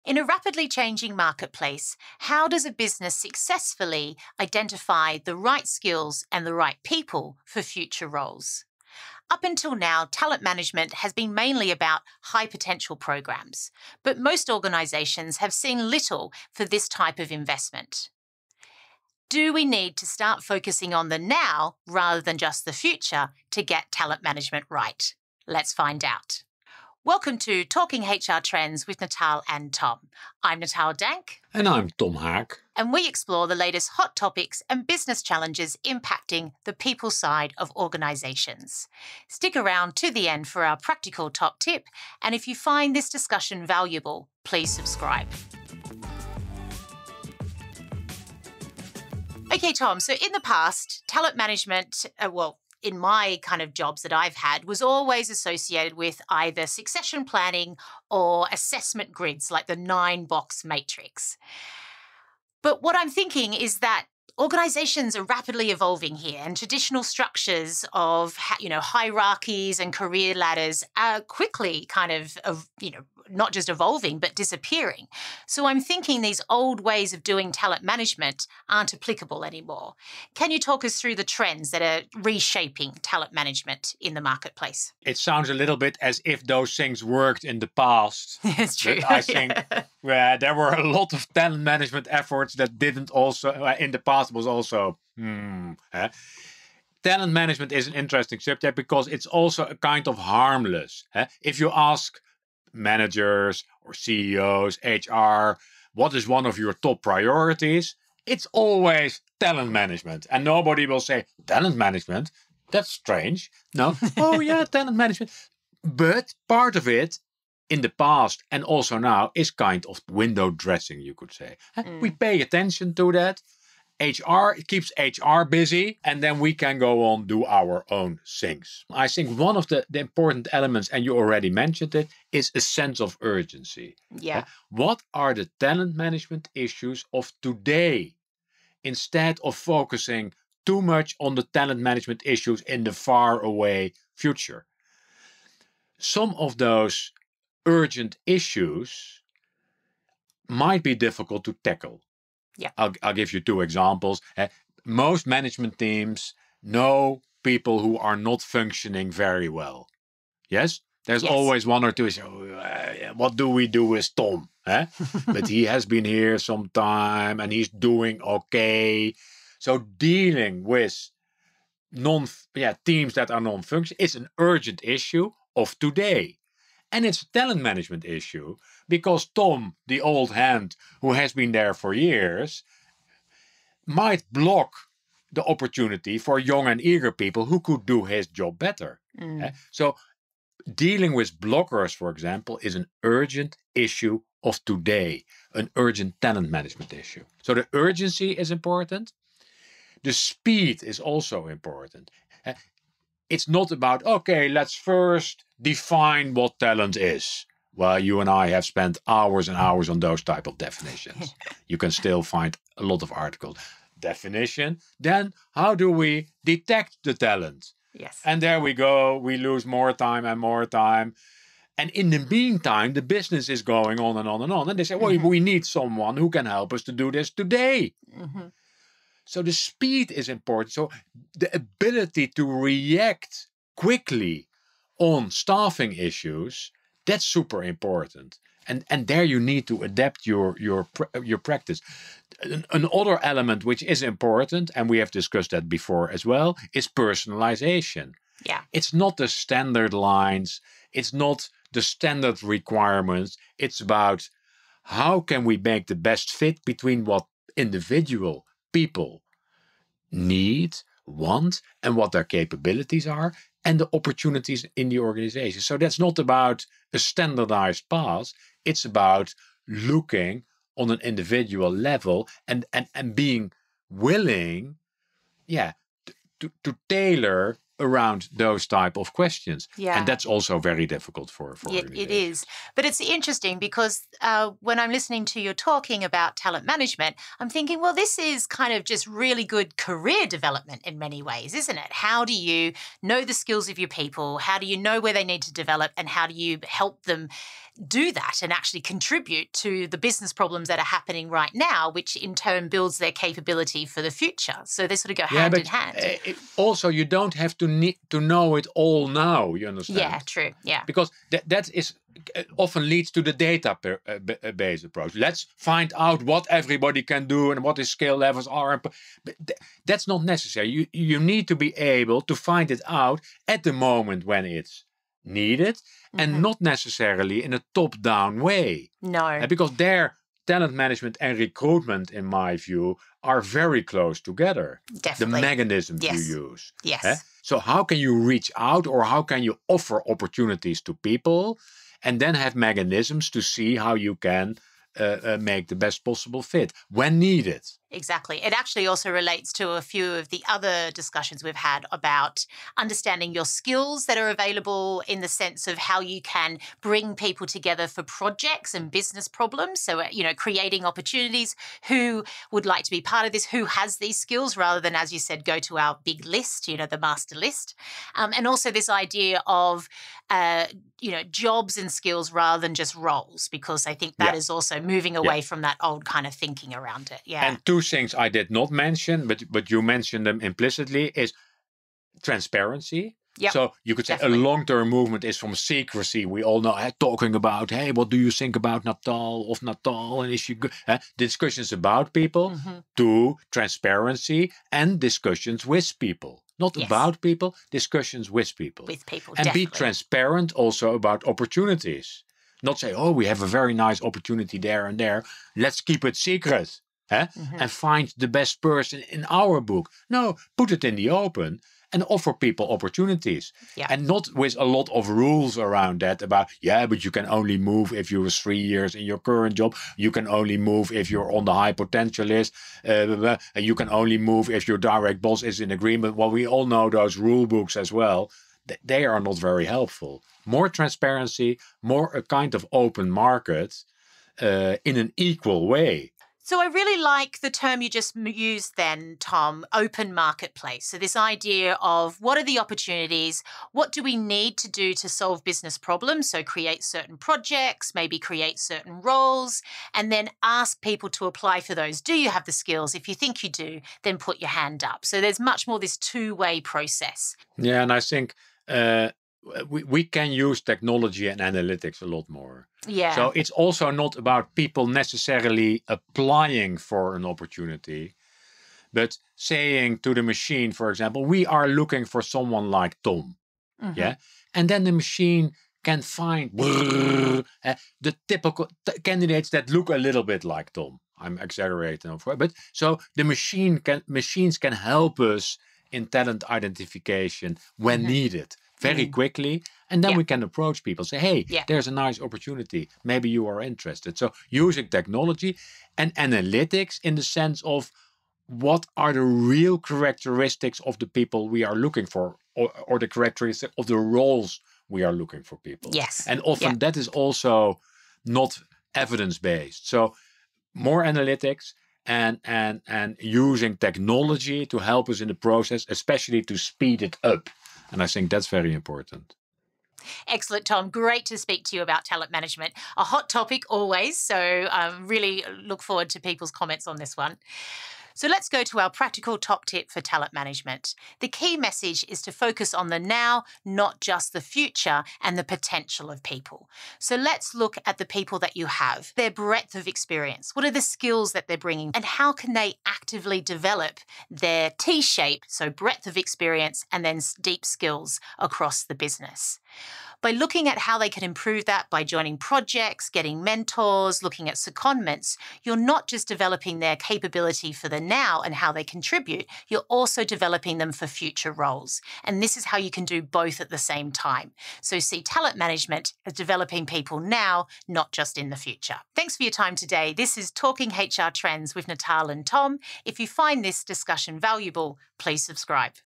Unboxing Talent Management
Talent management is always high on the priority list of organisation. In practice talent management processes are too much top-down and too slow.  Natal Dank of PXO Culture and Tom Haak of the HR Trend Institute discuss some alternatives in this episode of "Talking HR Trends with Natal & Tom".

Sections:

0:00 Introduction
0:48 What are the trends that are reshaping talent management?
6:16 The need for transparency
7:15 An open marketplace for talent
9:34 Practical Top Tip